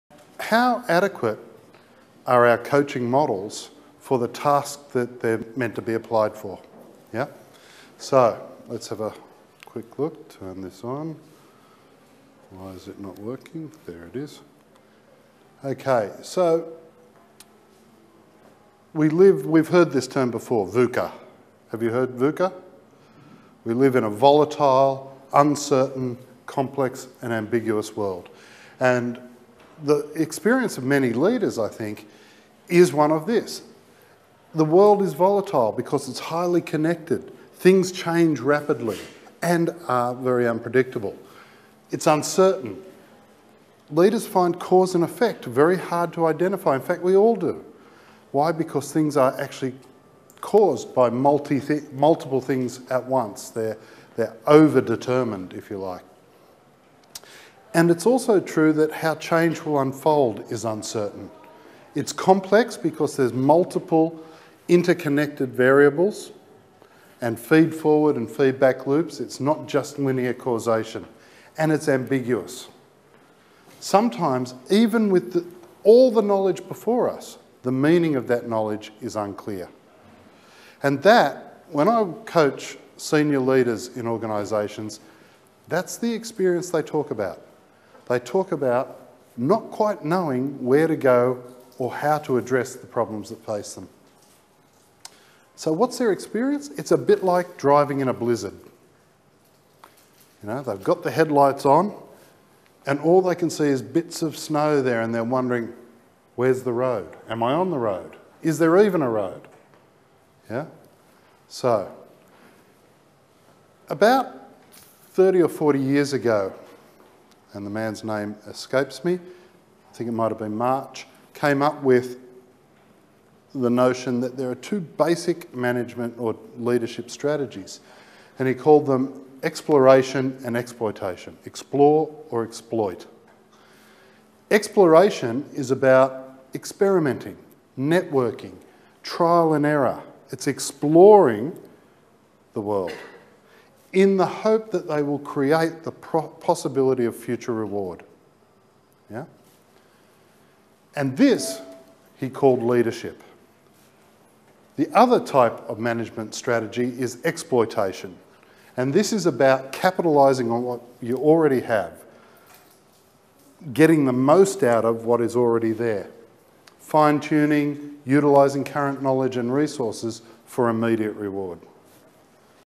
Dr. Michael Cavanagh - Growing the coaching field, and finding our place within it...
Dr. Michael Cavanagh: Growing the coaching field, and finding our place within it: Understanding what we bring and what we need, presented at the 2nd International Congress for Cognitive Behavioral Coaching.

This is a small preview trailer. To view & buy the full version of the video please